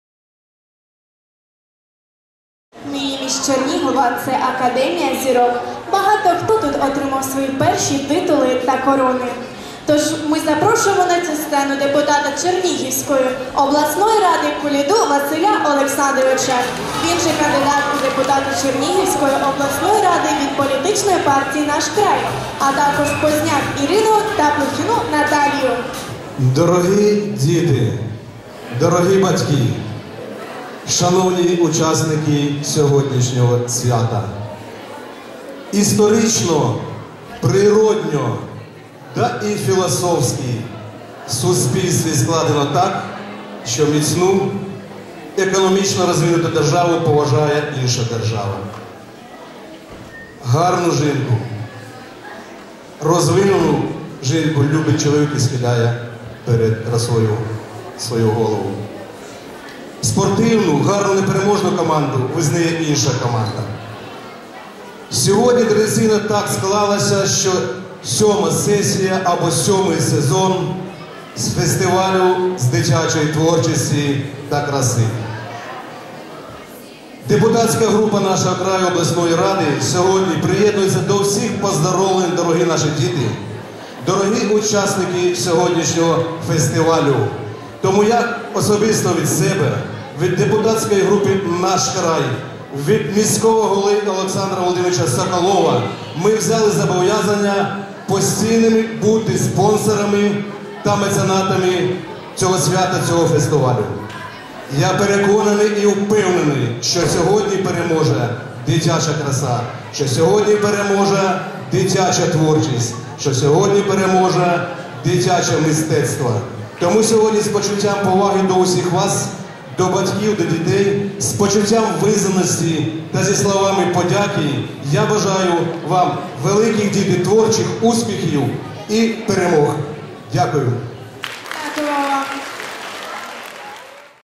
КТО "АКАДЕМИЯ ЗВЕЗД" "МИНИ МИСС ЧЕРНИГОВА 2015" ЧАСТЬ 3
О "Академия Звезд" представляет ежегодный благотворительный детский областной фестиваль красоты и таланта "Мини Мисс Чернигова и Детская Модель Чернигова, 2015" и конкурс первый  "Две Звезды, 2015" под руководством Янитты Мушинской и Марии Панкратьевой. Часть 3.